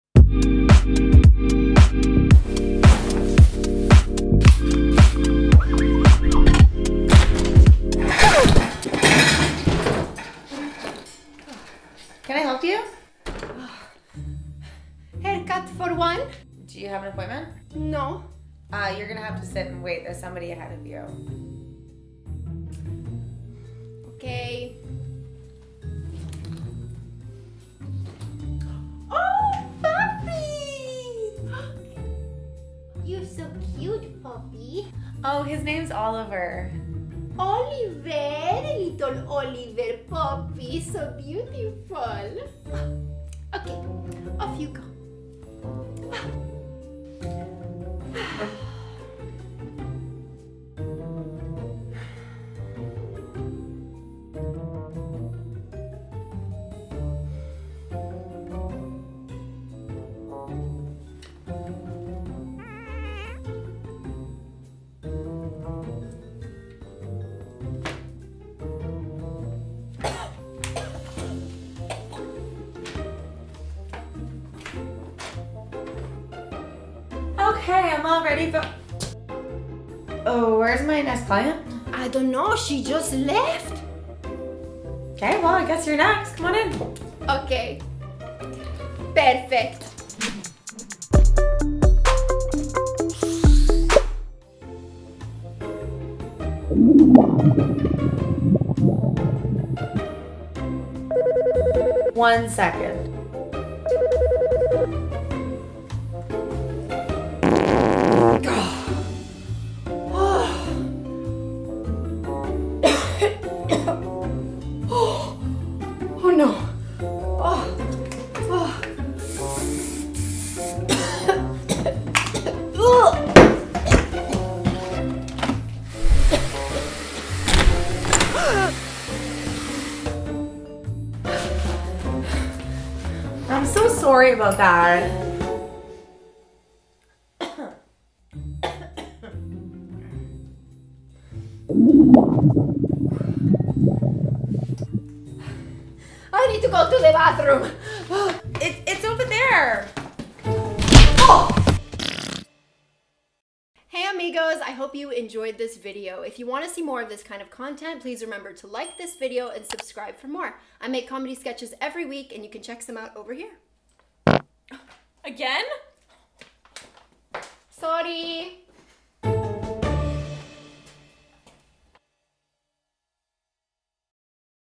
Farting in Public | Hair Salon Farts | Comedy Sketch
Gavi goes for a haircut, and comes out with a lot more problems...
Gassy Gavi strikes again!